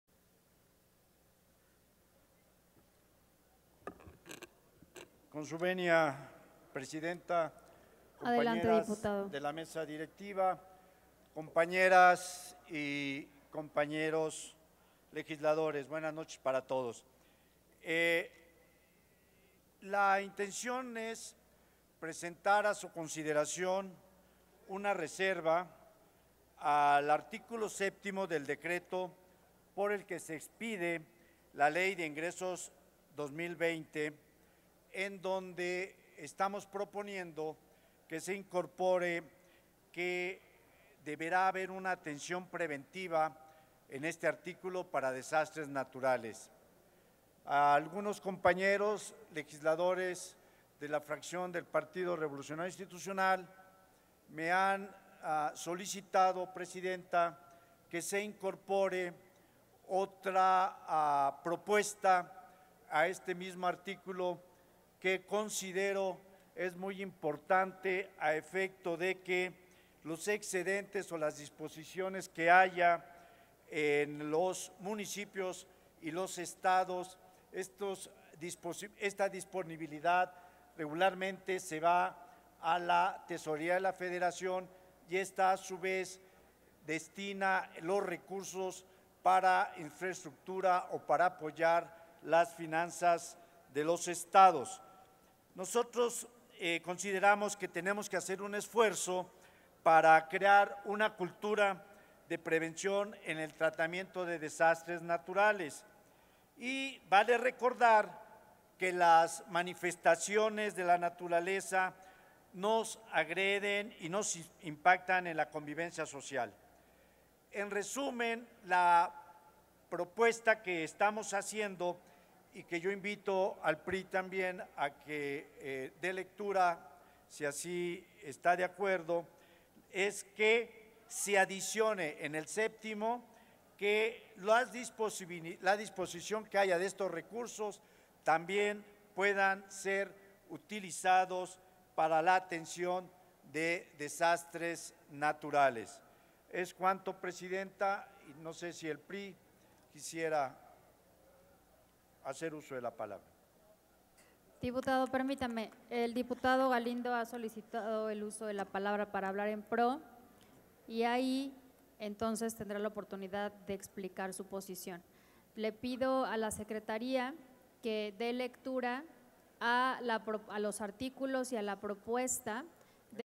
17/10/2019 Intervención desde tribuna del Dip. Raúl Eduardo Bonifaz Moedano
Intervención desde tribuna del Dip. Raúl Eduardo Bonifaz Moedano para presentar reserva al artículo Séptimo Transitorio al dictamen con proyecto de Ley de Ingresos de la Federación para el Ejercicio Fiscal de 2020.